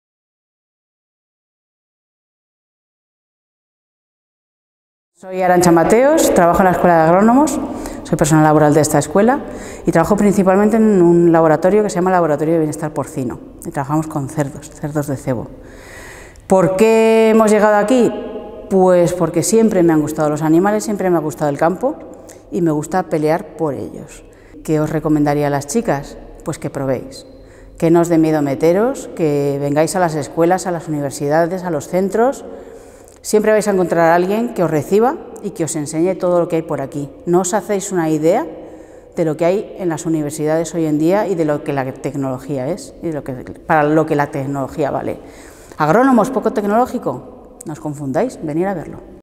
Día Internacional de la Mujer y la Niña en la Ciencia. Arancha Mateos San Juan
Arancha Mateos San Juan es Personal de Administración y Servicios (PAS) del departamento de Ingeniería Agroforestal de la ETSIAAB. Concretamente, trabaja como técnica en el Laboratorio de Bienestar Porcino donde se desarrolla –y disfruta– profesionalmente.